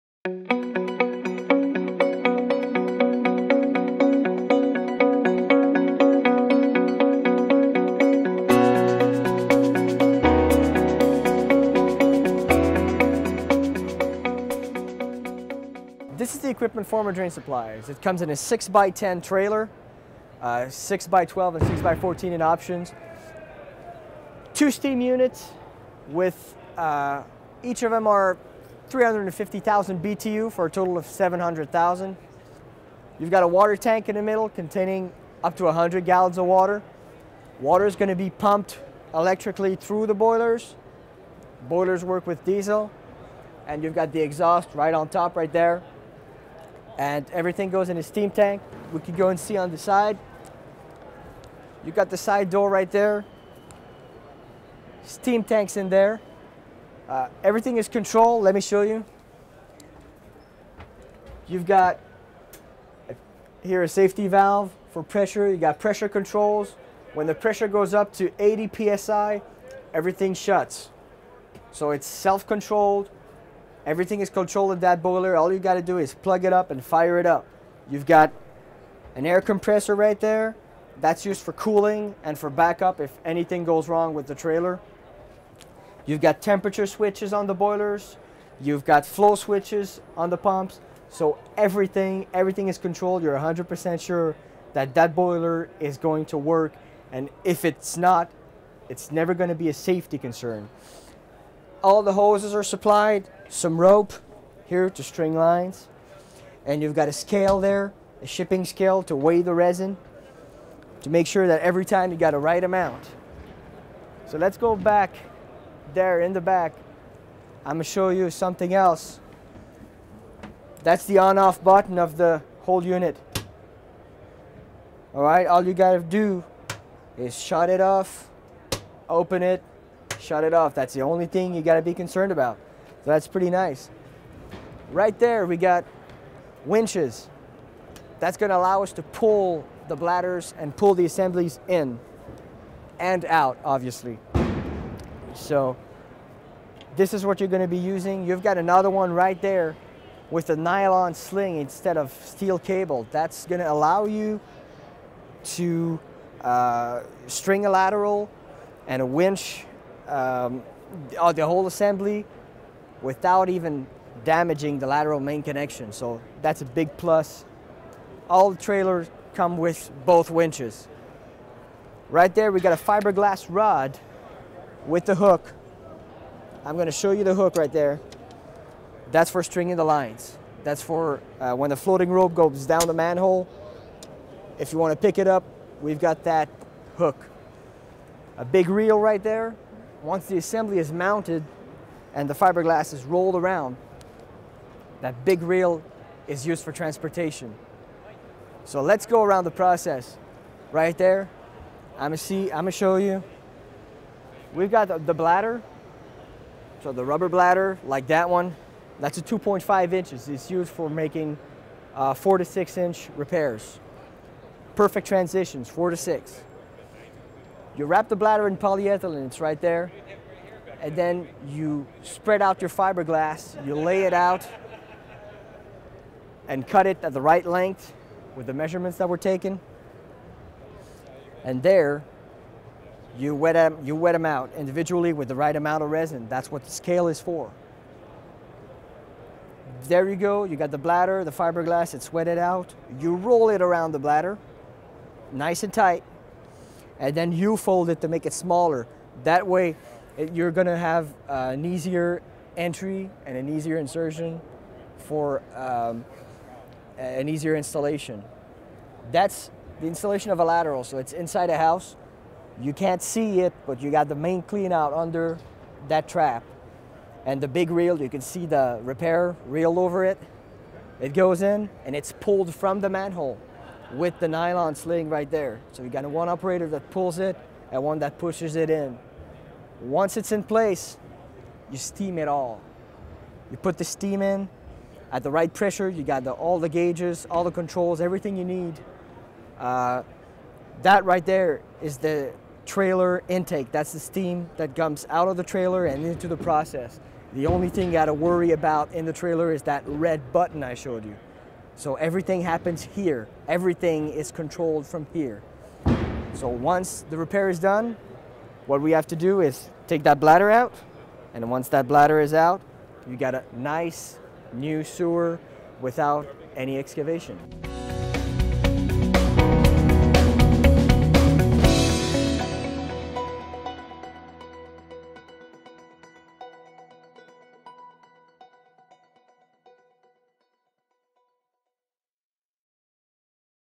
Formadrain No Dig System Trailer Package
A thorough look at Formadrain's No Dig pipe repair system including the trailer each licensee receives, all the contents from boiler to winches and a quick look at how the Formadrain cured in place pipe repair system works.

for a free information kit.